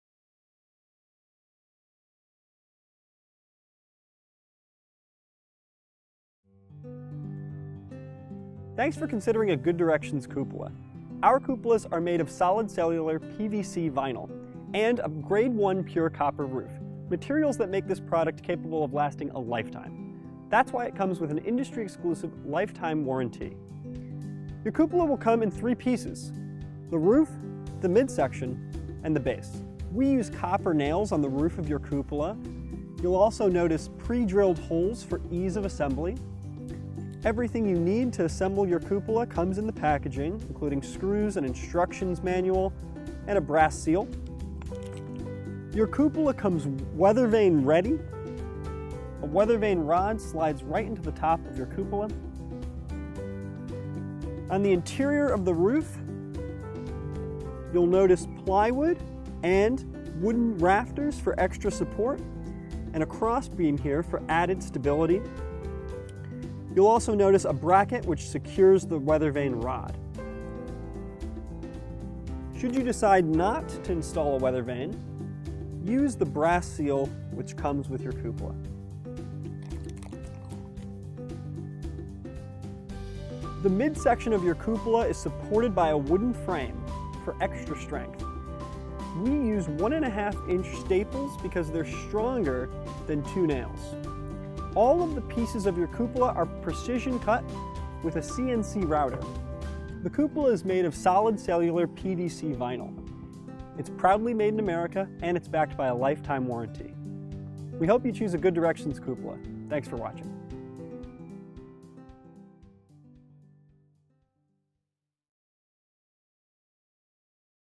Good Directions Viyl Cupolas
For over 35 years Good Directions cupolas have been the perfect complement to your home. Our expertly crafted, made to order, cupolas feature timeless American styling. It's made to order in the USA from premium-grade PVC for a lifetime of enjoyment.